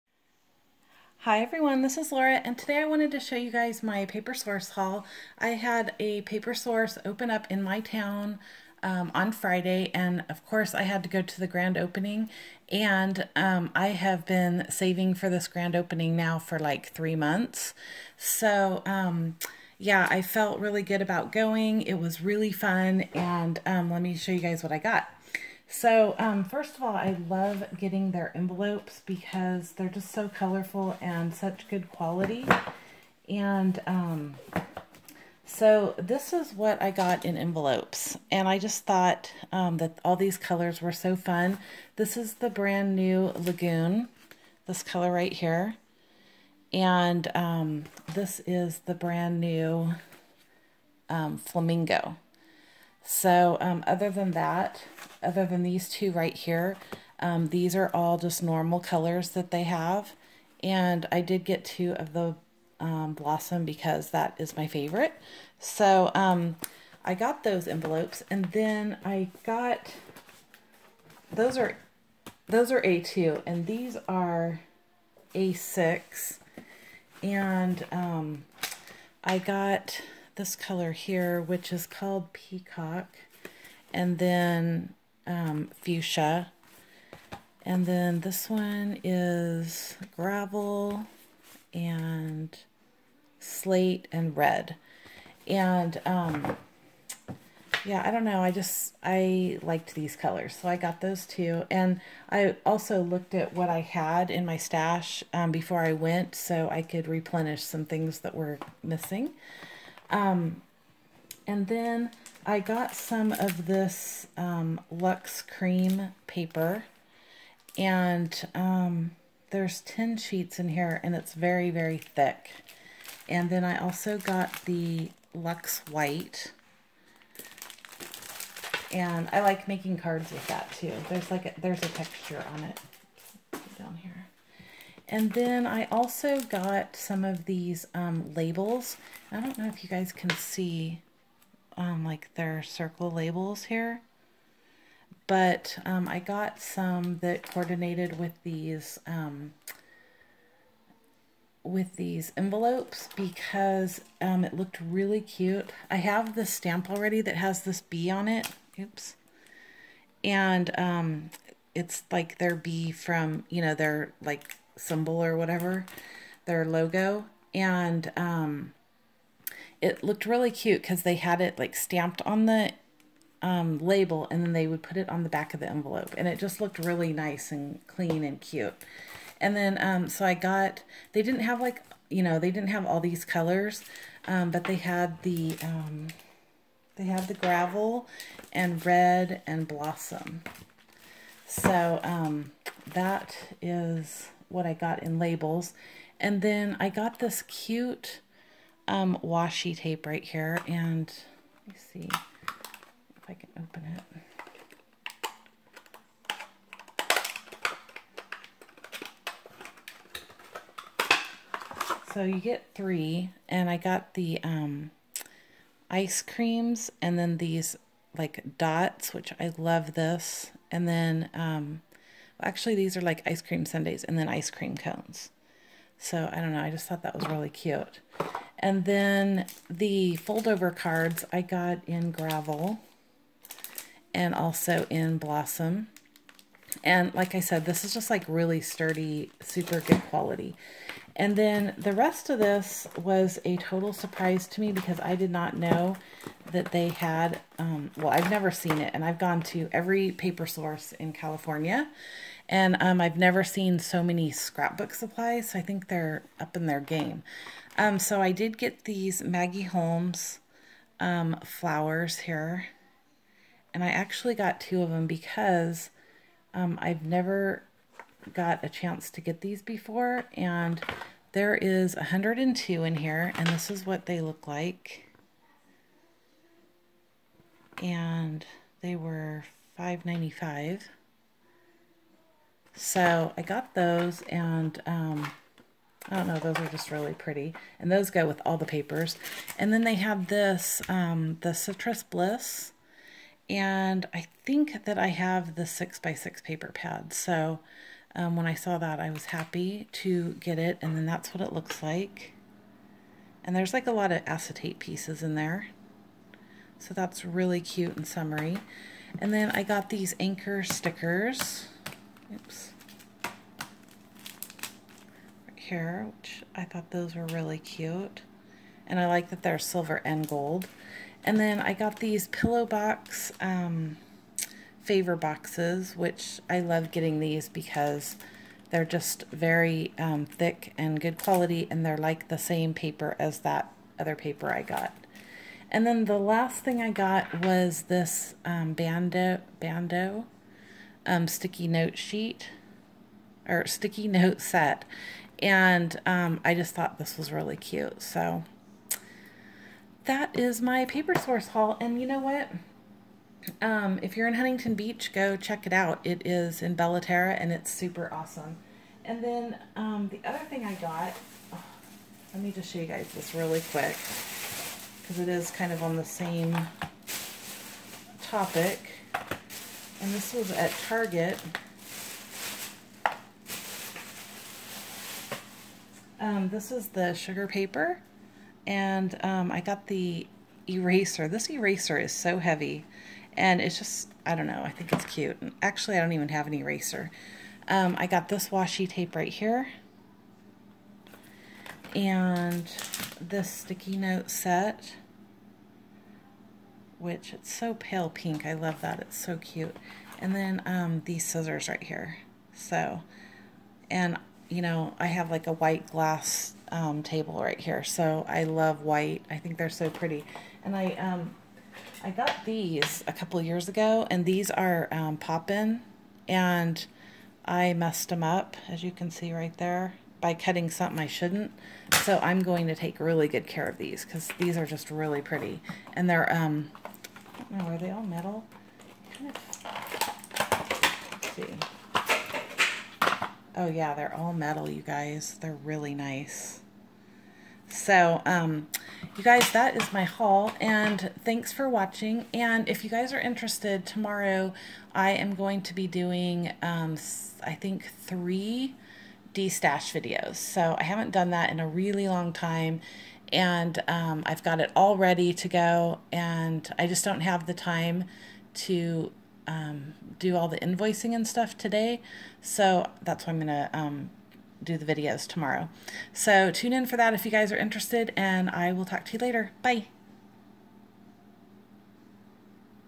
❤Paper-Source Haul❤
Some things I picked up at the Paper Source Grand Opening in Bella Terra. Also some Sugar Paper from Target:)  tfw